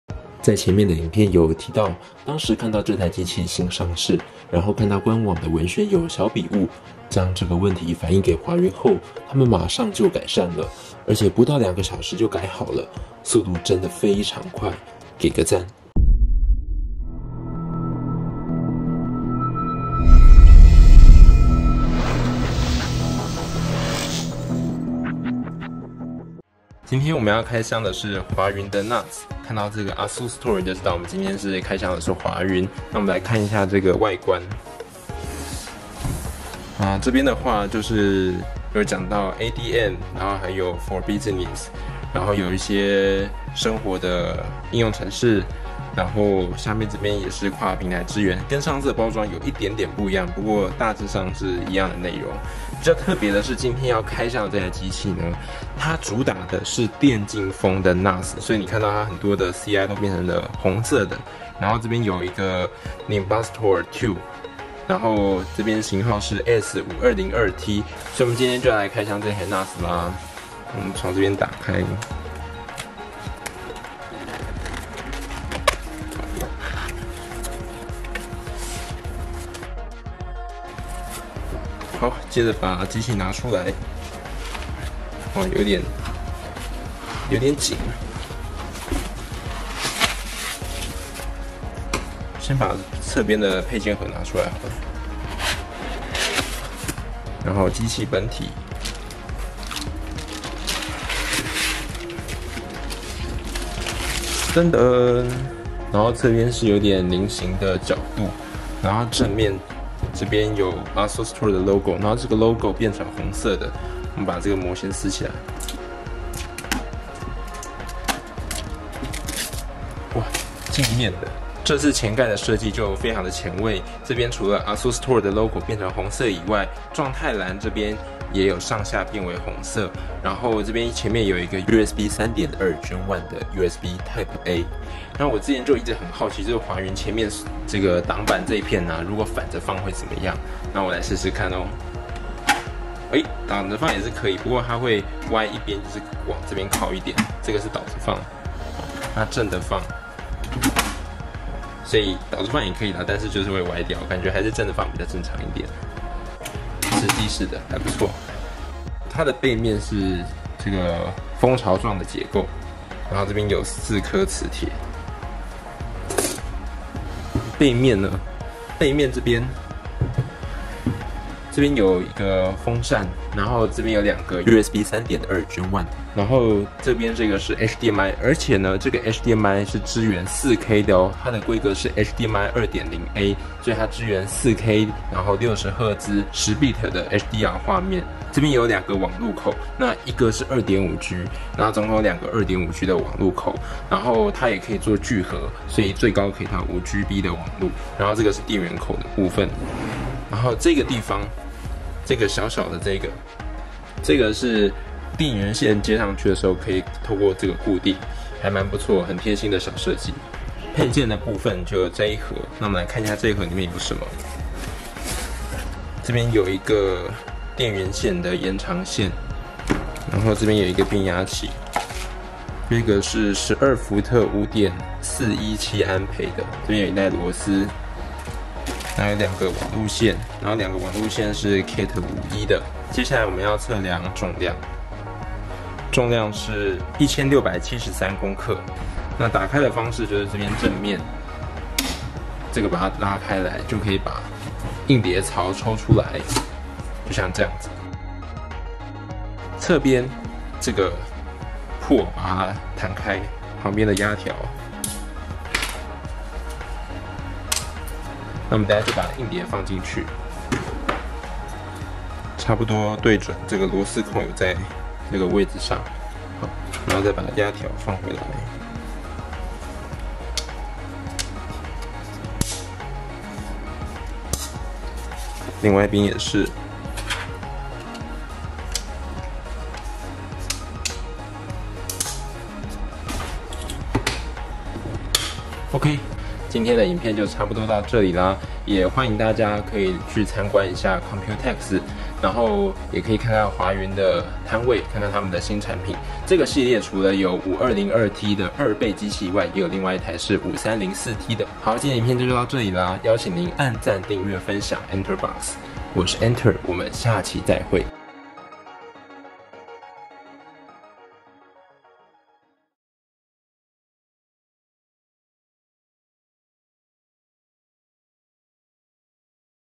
【開箱】ASUSTOR 電競魂 NAS AS5202T｜HDMI 4K 60Hz HDR 輸出
接續上次影片看到華芸新推出的 AS5304T 和 AS5202T，今天居然就要來開箱實機啦！從外箱就發現這次加入大面積紅色，相當顯眼。本體 AS5202T 機身上也有紅色的點綴，後置兩個 2.5GbE 的網路口也是紅色，讓產品更活潑動感。而且這次的 HDMI 是支援 4K、60Hz、HDR 的規格呢～想看電競 NAS 實機展示，可以到 COMPUTEX 的華芸攤位喔！